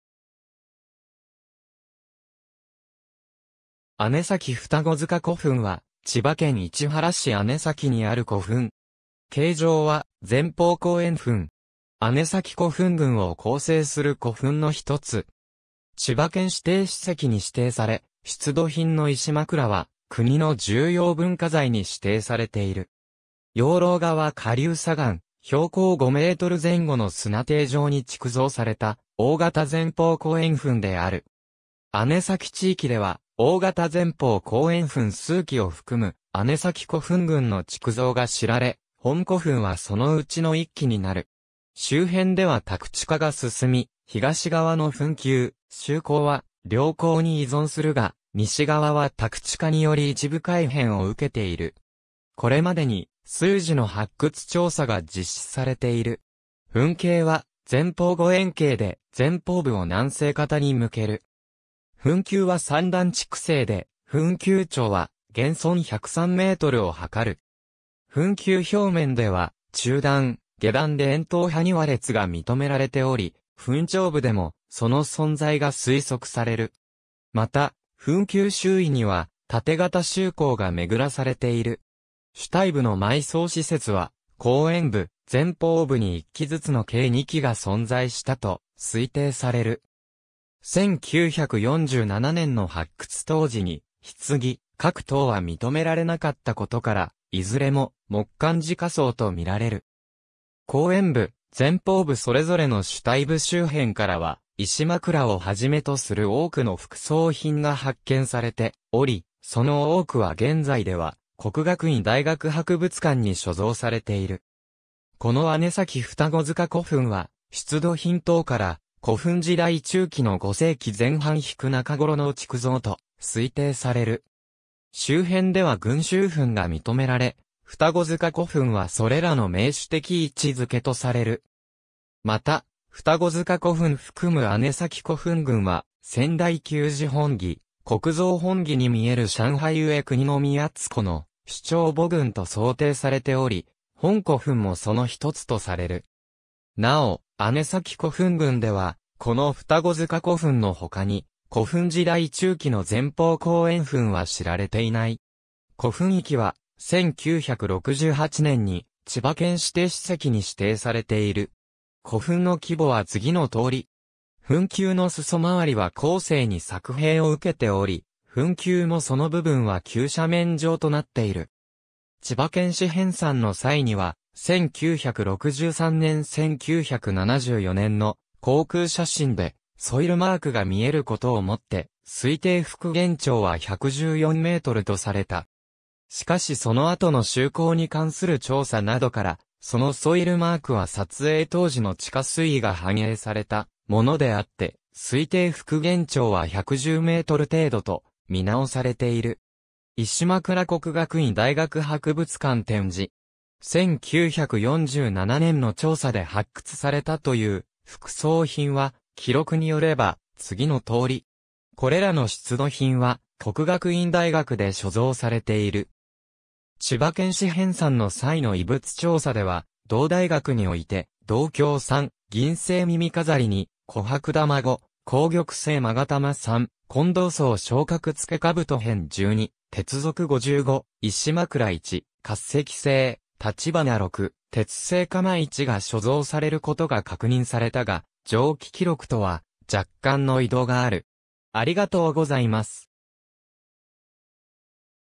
姉崎二子塚古墳
姉崎二子塚古墳（あねさきふたごづかこふん）は、千葉県市原市姉崎にある古墳。
姉崎古墳群を構成する古墳の1つ。
千葉県指定史跡に指定され（指定名称は「二子塚古墳」）、出土品の石枕は国の重要文化財に指定されている。
養老川下流左岸、標高5メートル前後の砂堤上に築造された大型前方後円墳である。
姉崎地域では大型前方後円墳数基を含む姉崎古墳群の築造が知られ、本古墳はそのうちの1基になる。
周辺では宅地化が進み、東側の墳丘・周溝は良好に遺存するが、西側は宅地化により一部改変を受けている。
これまでに数次の発掘調査が実施されている。
墳形は前方後円形で、前方部を南西方に向ける。
墳丘は3段築成で、墳丘長は現存103メートル（推定復原110メートル）を測る。
墳丘表面では中段・下段で円筒埴輪列が認められており、墳頂部でもその存在が推測される。
また墳丘周囲には盾形周溝が巡らされている。
主体部の埋葬施設は、後円部・前方部に1基ずつの計2基が存在したと推定される。
1947年（昭和22年）の発掘当時に棺・槨等は認められなかったことから、いずれも木棺直葬と見られる。
後円部・前方部それぞれの主体部周辺からは、石枕（国の重要文化財）を始めとする多くの副葬品が発見されており、その多くは現在では國學院大學博物館に所蔵されている。
この姉崎二子塚古墳は、出土品等から古墳時代中期の5世紀前半-中頃の築造と推定される。
周辺では群集墳が認められ、二子塚古墳はそれらの盟主的位置づけとされる。
また、二子塚古墳含む姉崎古墳群は『先代旧事本紀』「国造本紀」に見える上海上国造の首長墓群と想定されており、本古墳もその1つとされる。
なお姉崎古墳群では、この二子塚古墳のほかに古墳時代中期の前方後円墳は知られていない。
古墳域は、1968年（昭和43年）に千葉県指定史跡に指定されている。
古墳の規模は次の通り（値は推定復原）。
墳丘の裾周りは後世に削平を受けており、墳丘もその部分は急斜面状となっている。
千葉県史編纂の際には、1963年（昭和38年）・1974年（昭和49年）の航空写真でソイルマークが見えることをもって推定復原長は114メートルとされた。
しかしその後の周溝に関する調査などから、そのソイルマークは撮影当時の地下水位が反映されたものであって、推定復原長は110メートル程度と見直されている。
石枕（国の重要文化財）國學院大學博物館展示。
1947年（昭和22年）の調査で発掘されたという副葬品は、記録によれば次の通り。
これらの出土品は、國學院大學で所蔵されている。
千葉県史編纂の際の遺物調査では、同大学において銅鏡3、銀製耳飾り2、琥珀玉5、硬玉（翡翠）製勾玉3、金銅装衝角付冑片12、鉄鏃55、石枕1、滑石製立花6、鉄製鎌1が所蔵されることが確認されたが、上記記録とは若干の異同がある。